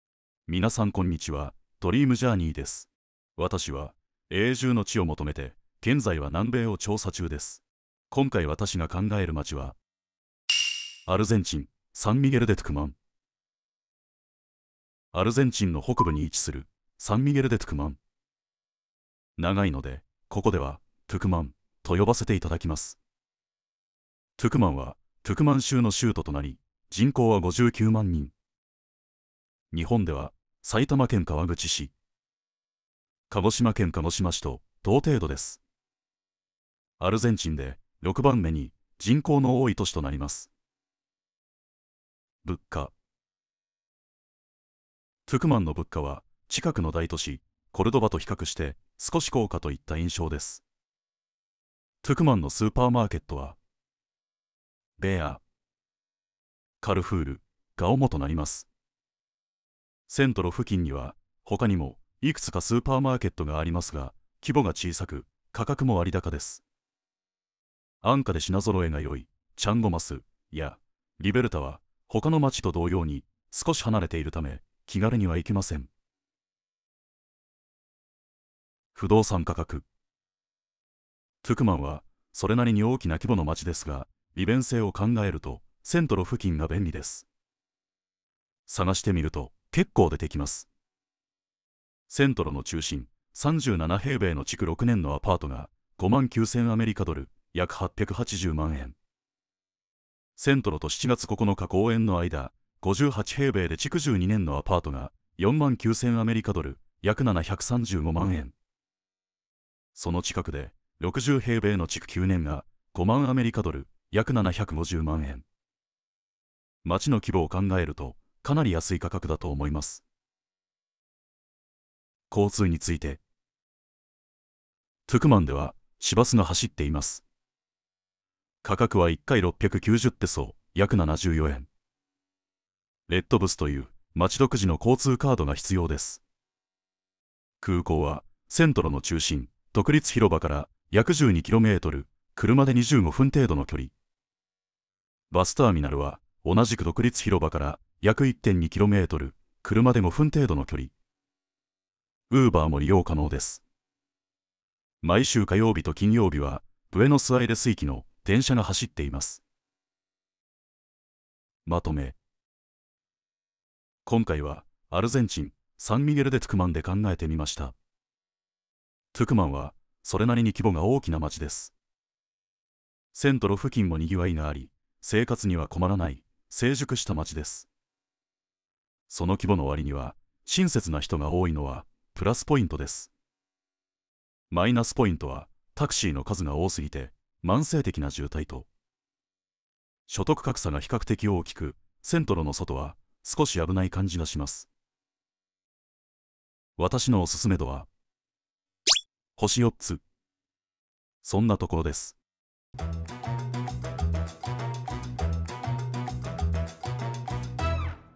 南米移住考察【アルゼンチン】サン・ミゲル・デ・トゥクマン編
南米、アルゼンチンのサン・ミゲル・デ・トゥクマンに移住するならどこに住むかを考えてみました

00:00 オープニング
00:15 街の紹介
00:42 物価
01:18 不動産価格
02:02 交通について
02:41 まとめ

動画の情報は2024年時点でのものです